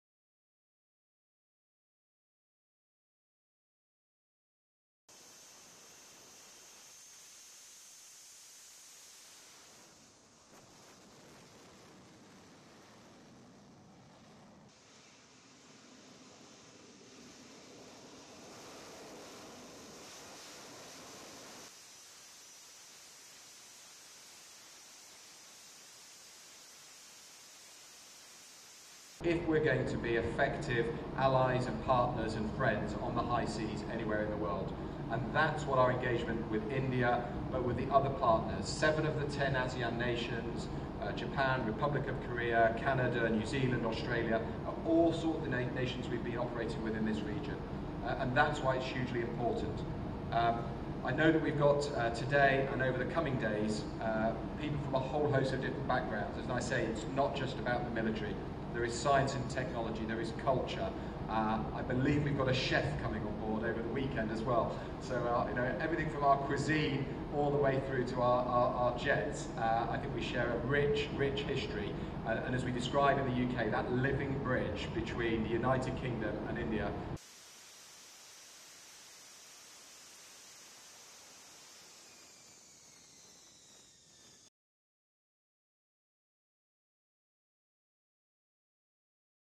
F-35B fifth generation fighter aircraft takes off from UK warship in Arabian Sea near Mumbai
Mumbai, Oct 21 (ANI): On October 21, F-35B fifth-generation fighter aircraft took off from the British Royal Navy aircraft carrier HMS Queen Elizabeth in the Arabian Sea near Mumbai. The warship is in India for a joint exercise with Indian forces.“This exercise is hugely important for relations with India. It's not just about defence, but also about the science & technology and relation between the 2 countries,” said Commodore Steve Moorhouse, British Navy. (SA)